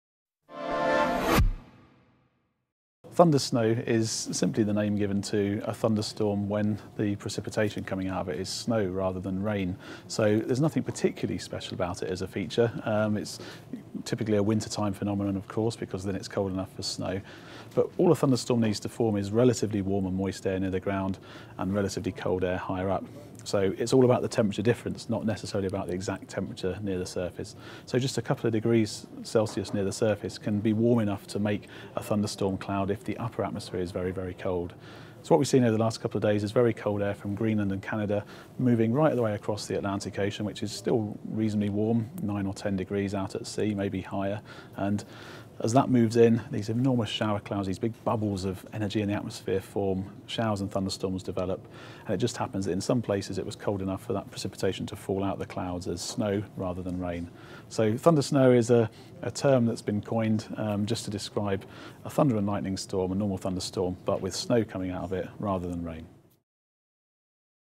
What is thundersnow?
What is the phenomenon known as thundersnow and how is it caused? Weather expert Paul Knightley from Meteogroup explains

Thundersnow has been reported in parts of the UK, with Cornwall, Wales and Teesside among the areas experiencing the dramatic-sounding storms.

But what are the characteristics of this weather event?
Thundersnow occurs when thunderstorms form in wintry conditions. "Thundersnow is simply the name given to a thunderstorm when the precipitation coming out of it is snow rather than rain," says forecast manager Paul Knightley.
The result is heavy downpours of snow, with thunder and lightning.

Thunder-snowstorms are quieter than regular thunderstorms as the noise is muffled by the snow. The lightning may also appear brighter, as the light reflects off the snowflakes.